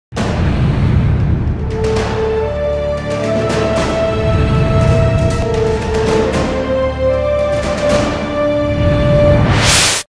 New Intro!
My brand new opening to my videos! Do you like this new intro?

P.S. Another early video!